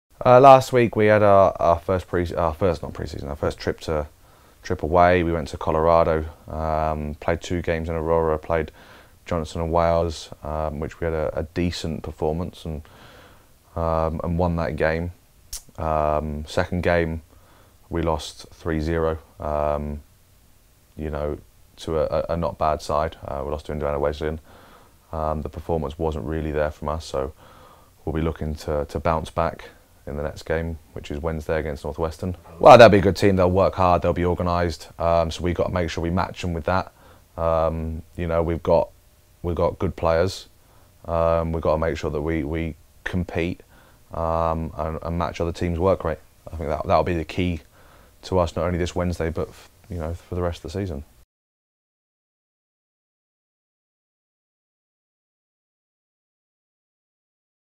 2017 Men's Soccer Weekly Recap - Week 2
Head men's soccer Mark Heath-Preston recaps the second week of the 2017 season and previews the upcoming week.